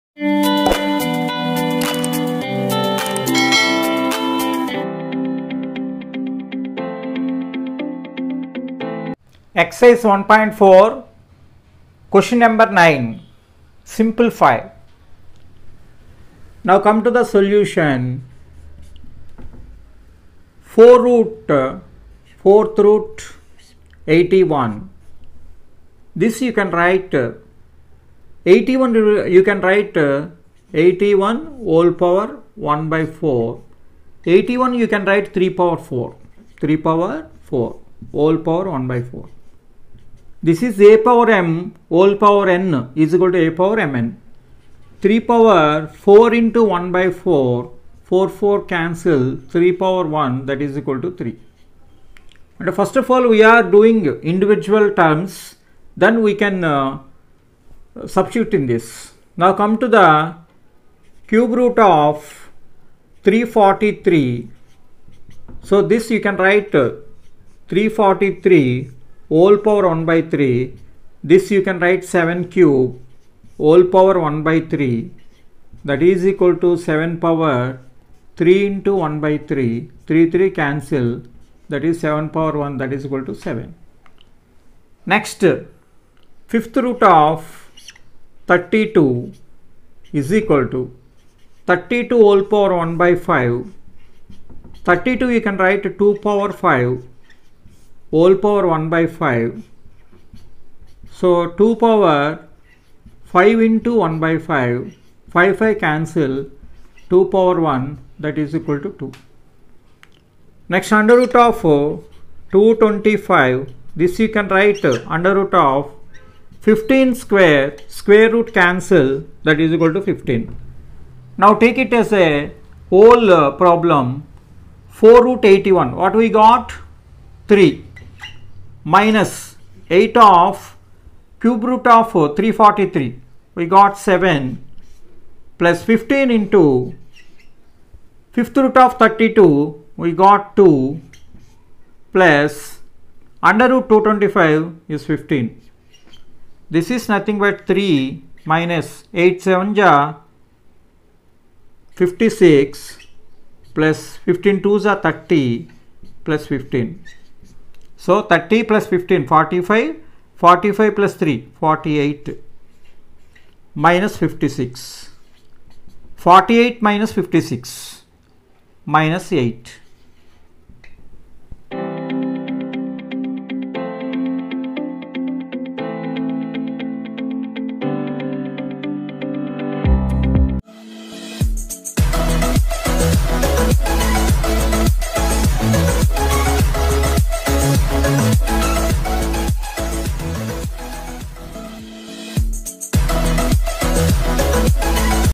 Class - 9th | Ex:-1.4 | Q.No:- 9 | Real numbers | AP | TS | Bum sir
This video:- Class - 9th | Ex:-1.4 Q.No:- 9 | Real numbers | AP | TS | Bum sir
Hey!
Students are facing problem while studying online So, here we are with a  video of Maths Class 9th First Chapter "real numbers" Ex:-1.4 Q.No:- 9 . We hope this will help you.